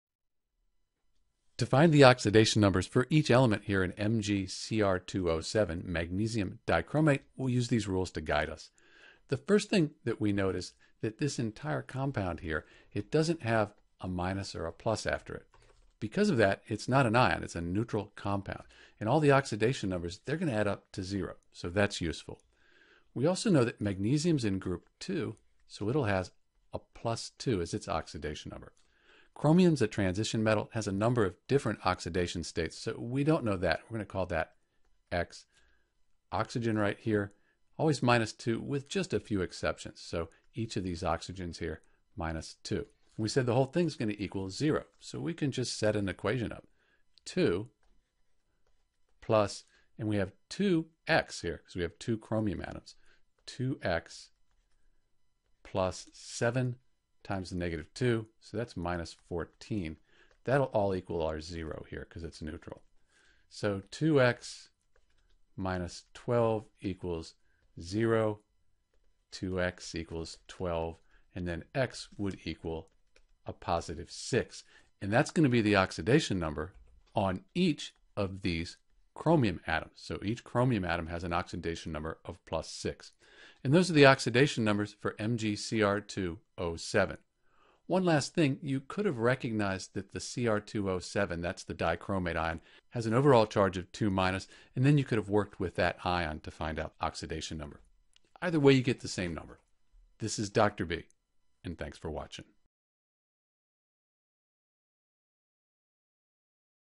How to find the Oxidation Number for Cr in MgCr2O7
To find the correct oxidation state of Cr in MgCr2O7 (Magnesium dichromate), and each element in the molecule, we use a few rules and some simple math.

First, since the MgCr2O7 molecule doesn’t have an overall charge (like NO3- or H3O+) we could say that the total of the oxidation numbers for MgCr2O7 will be zero since it is a neutral molecule.

We write the oxidation number (O.N.) for elements that we know and use these to figure out oxidation number for Cr.

Here it is bonded to Cr so the oxidation number on Oxygen is -2. Using this information we can figure out the oxidation number for the element Cr in MgCr2O7.